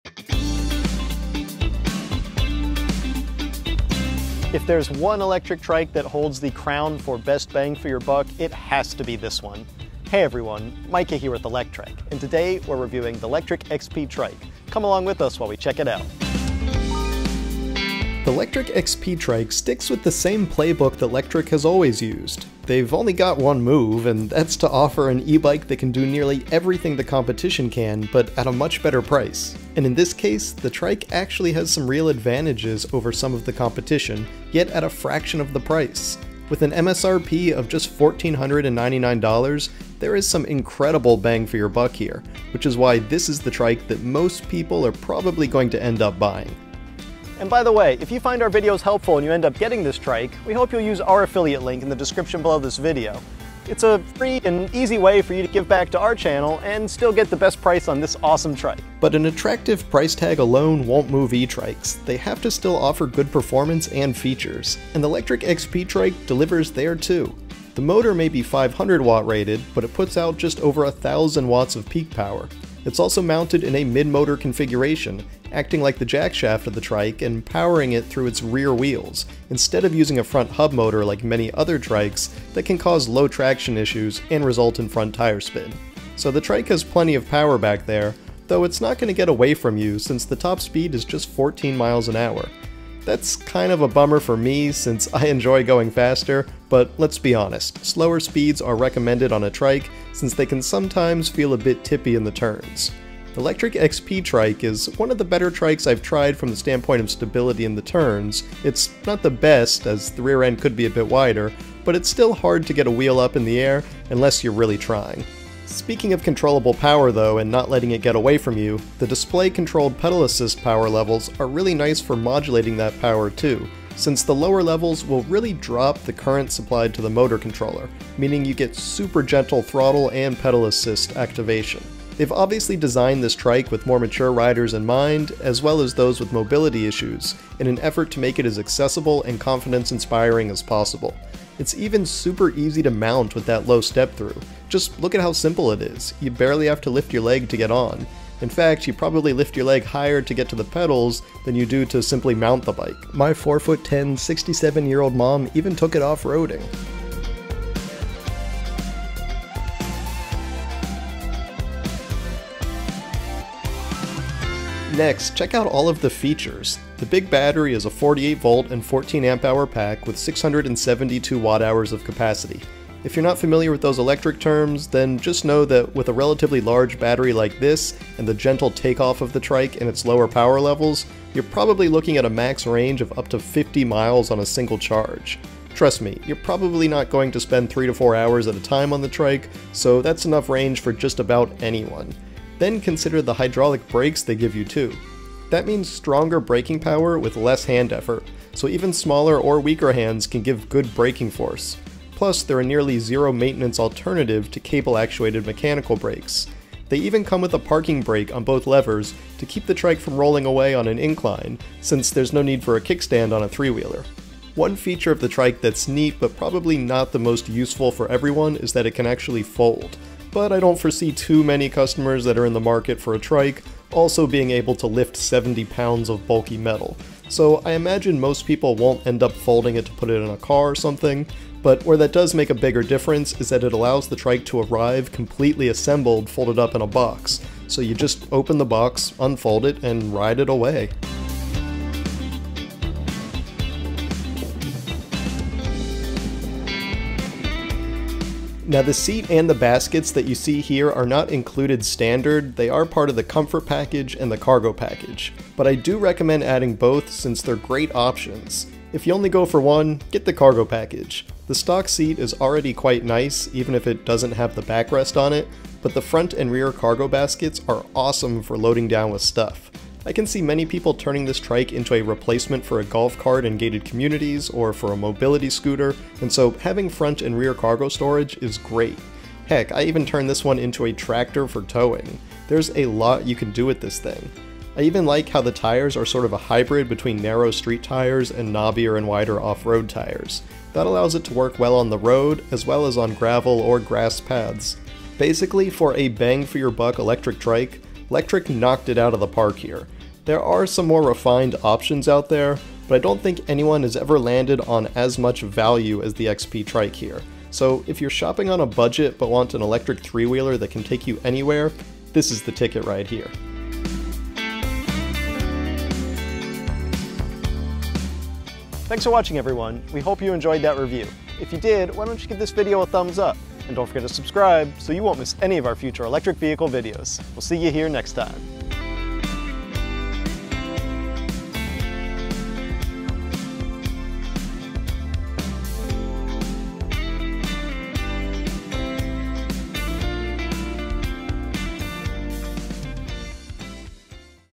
Lectric XP Trike review: Why you'll likely buy this e-trike
The Lectric XP Trike ($1,499 has the best bang for your buck of any other electric trike on the market. It's not perfect, but it's pretty darn great. Read all about this fun e-trike and my experience testing it over on Electrek

⛑️ My safety gear:
Bike helmet

🎥 My filming gear:
Pretty product shots
Invisible selfie stick camera
Chest mounted camera
Handlebar phone holder